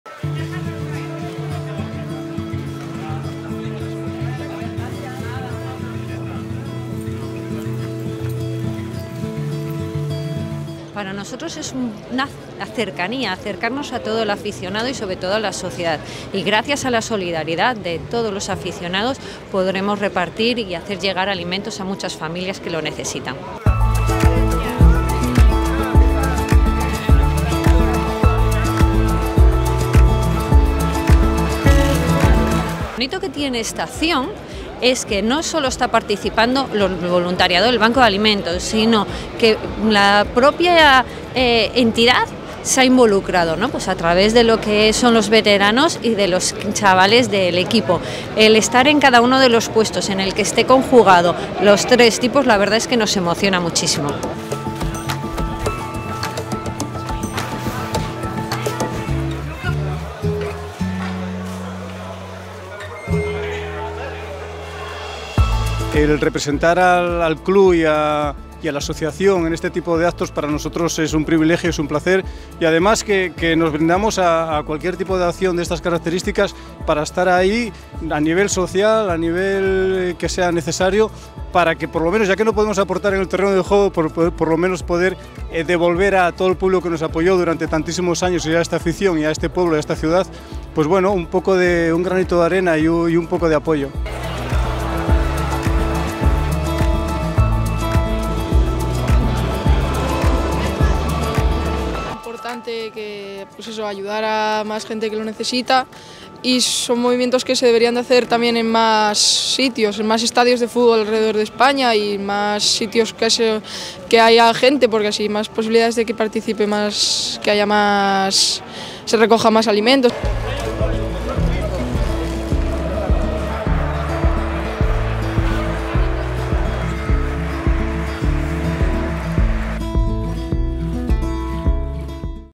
Recogida de alimentos en El Molinón
El Real Sporting de Gijón y el Banco de Alimentos de Asturias unieron sus fuerzas en la previa del partido de la jornada 14 para realizar una recogida de alimentos que fue un éxito.